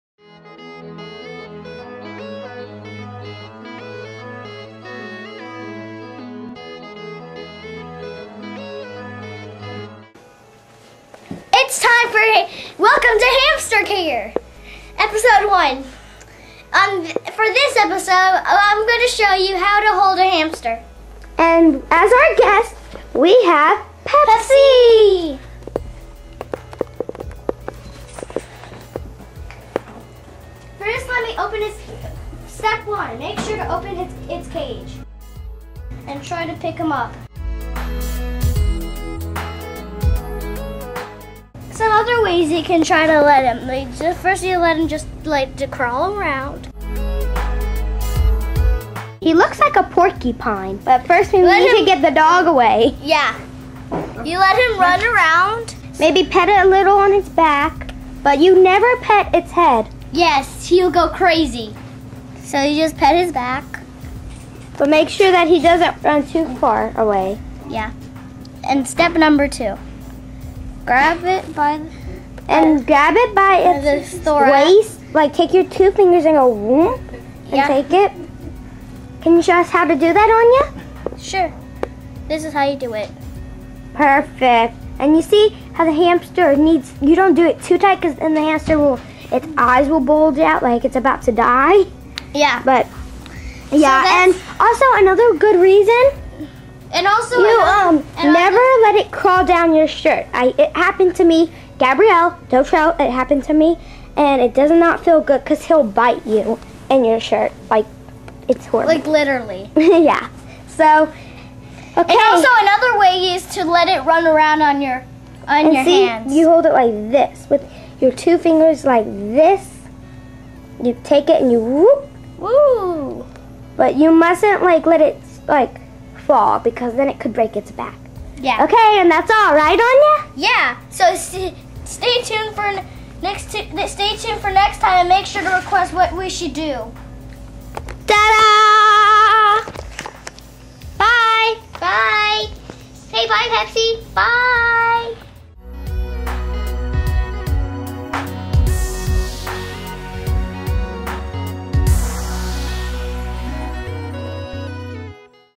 Hamster care episode 1
Anya and Gabrielle show how to hold a hamster